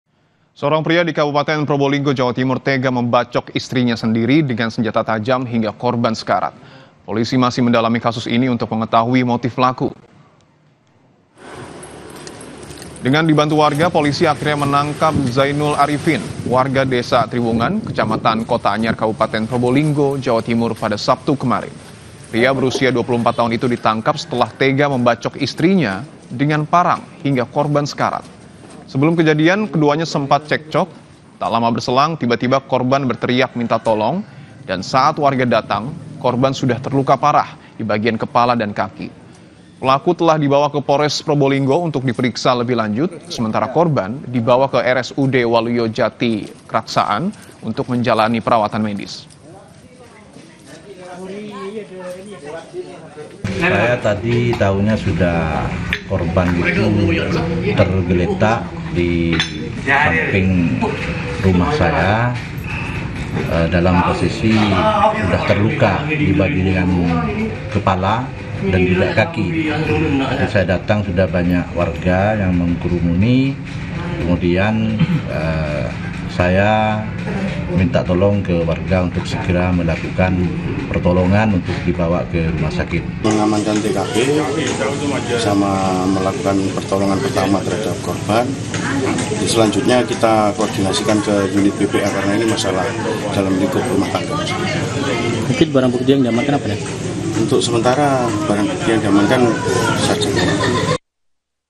Seorang Suami Di Probolinggo Tega Bacok Istri Sendiri
PROBOLINGGO, KOMPAS.TV - Seorang Suami Di Probolinggo Tega Bacok Istri Sendiri